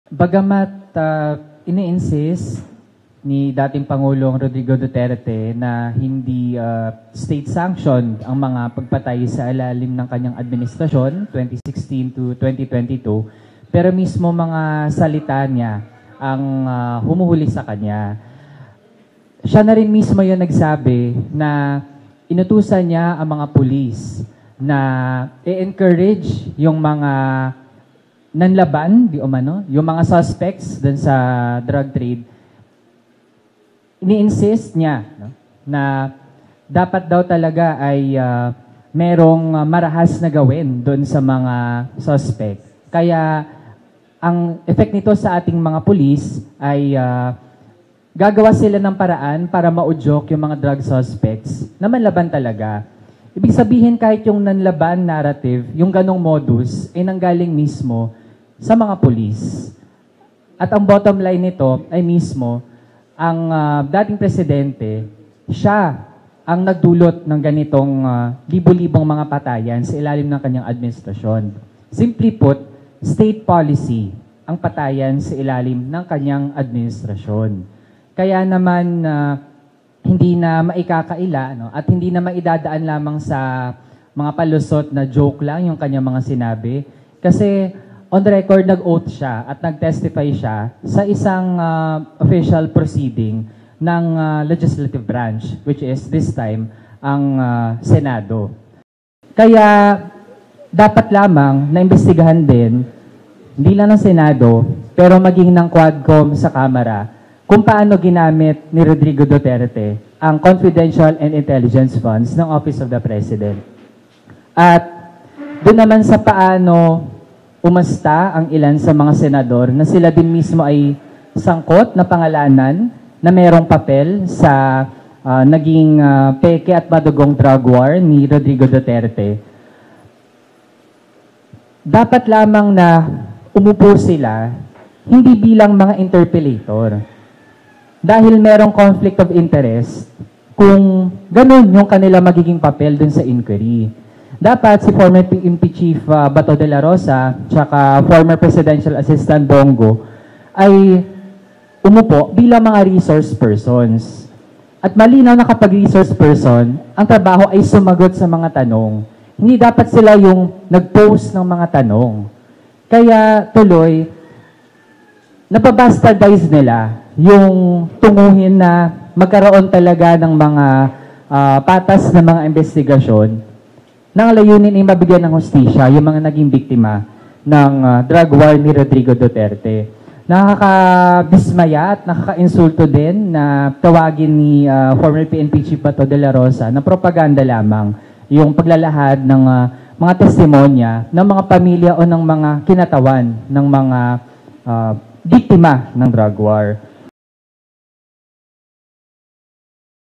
Rep. Raoul Manuel: Sens. Bato Dela Rosa at Bong Go, dapat umupo bilang resource person
News5OnTape | Binigyang diin ni Kabataan Party-list Rep. Raoul Manuel na dapat umupo bilang resource persons sina Sen. Bato Dela Rosa at Sen. Bong Go dahil "naba-bastardize" umano ang pagdinig hinggil sa war on drugs ng administrasyong Duterte sa pagkakaroon ng patas na imbestigasyon na layunin ay makapagbigay ng hustisya.